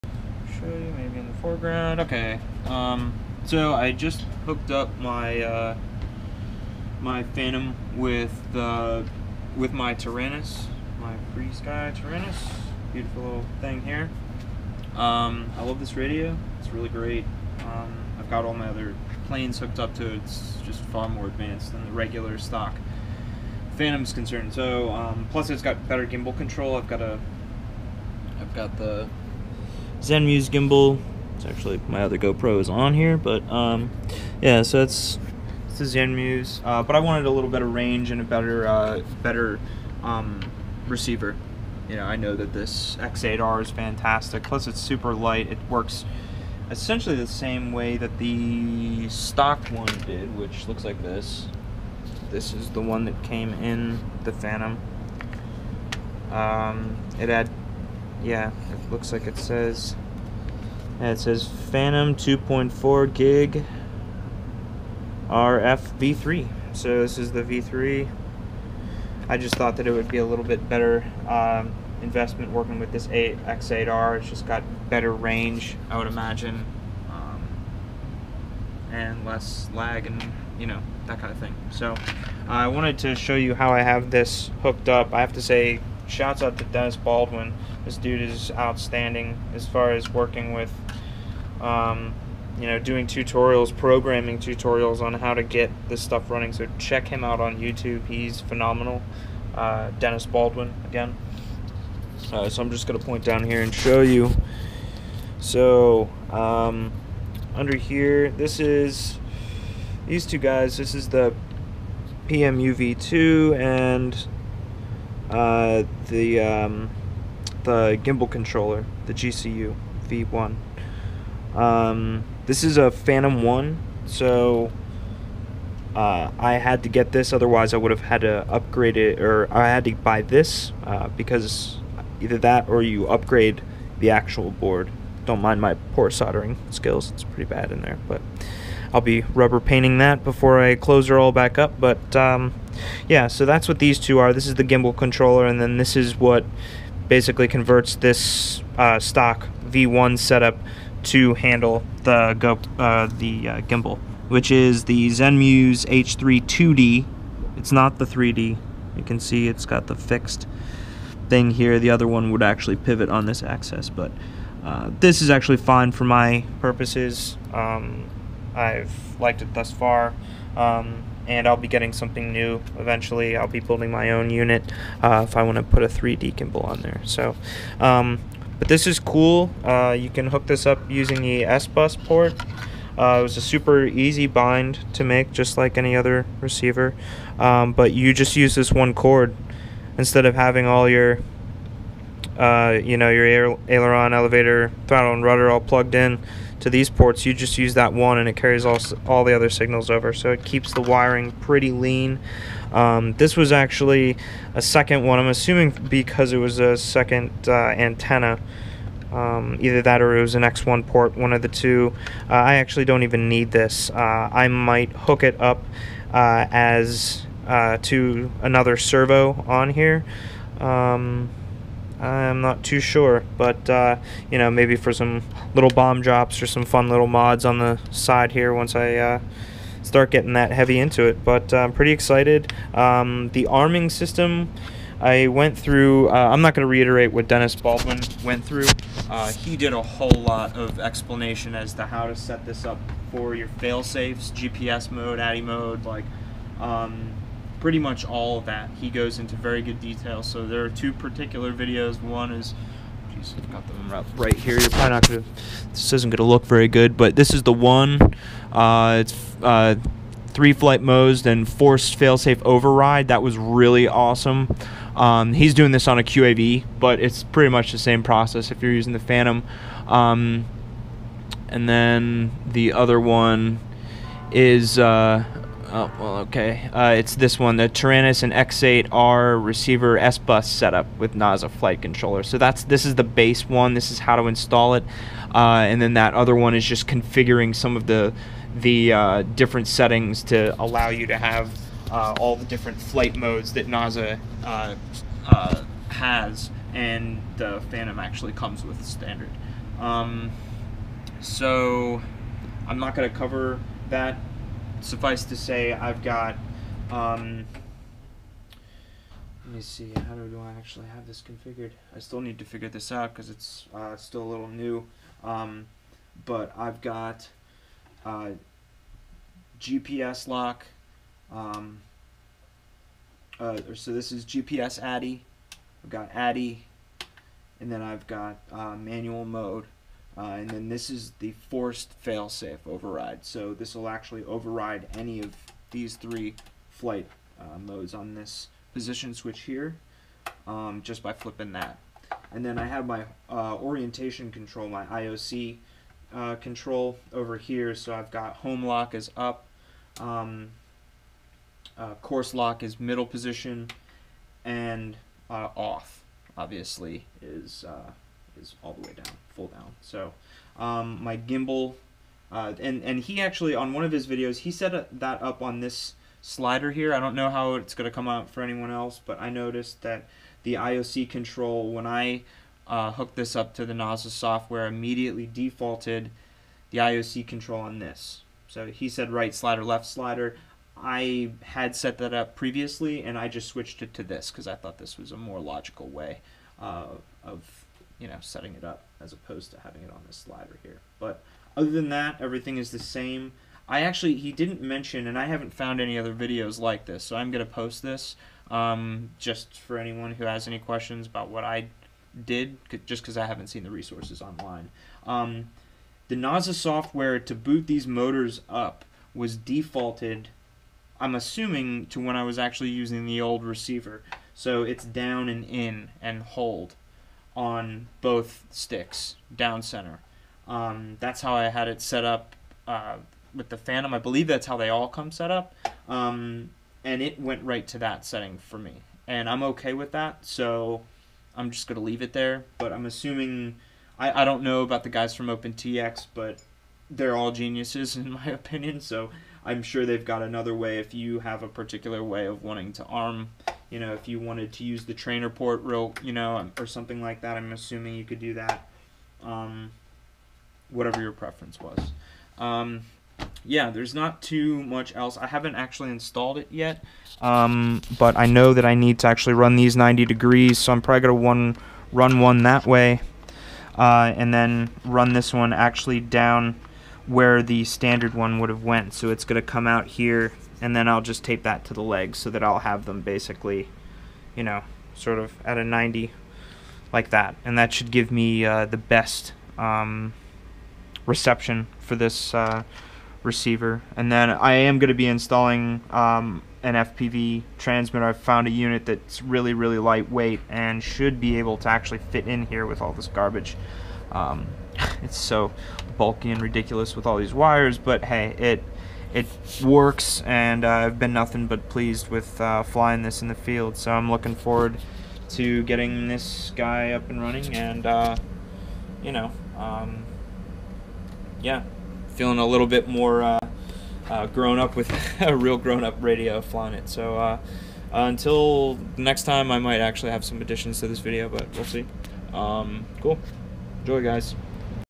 Phantom V.1.1.1 w/ FrSky X8R & FrSky Taranis
I know this is super old news, but I figured I'd post for anyone who has the same gear.

I have my Phantom v. 1.1.1. bound with a FrSky X8R Rx and my Taranis X9D using the SBUS port. This is how the Phantom comes set up with its stock Rx, and you can use the same servo cable to the X8R. Naza configuration was very easy, same config procedure as DJI's transmitter. Let me know if anyone is trying to get this to work, but can't, and I'll try and help.

Links to the videos I watched to get my setup working...Thanks Dennis Baldwin!

Note: I have this Phantom as an archived firmware, and do not plan to upgrade. (Its working fine, no need to change it, IMO.)